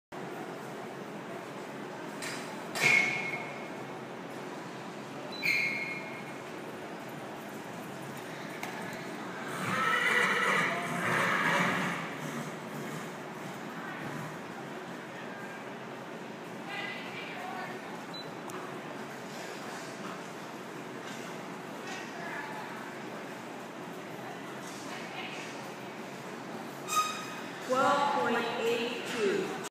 Shannon Mcbride and dotted jaguar goat tying freshman year
HS rodeo Labor Day weekend in cannon falls